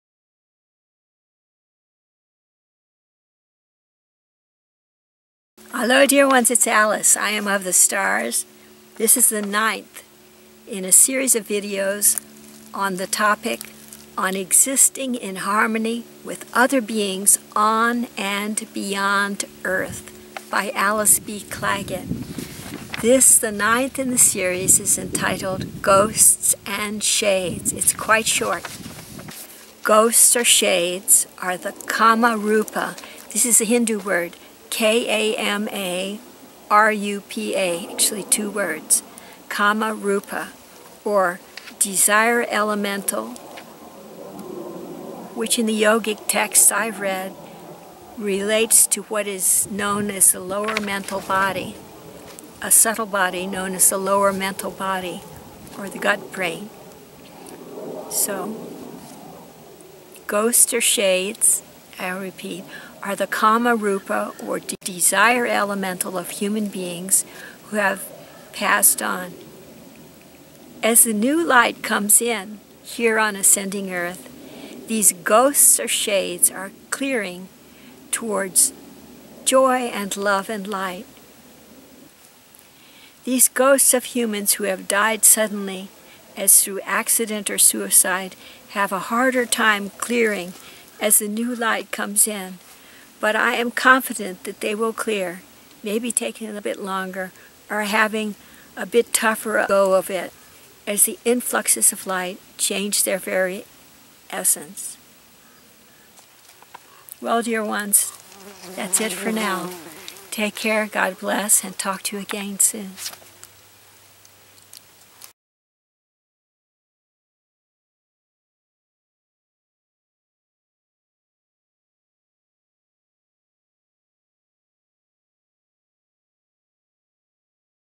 Harmony with Other Beings 9. Ghosts and Shades . by Alice B. Clagett
Written and published on 21 August 2016; filmed on 8 September 2021; film published on 13 September 2021
FULL Title: On Existing in Harmony with Other Beings on and Beyond Earth: 9. Sentient Astral Beings - Ghosts and Shades . channeled by Alice B. Clagett
Dear Ones,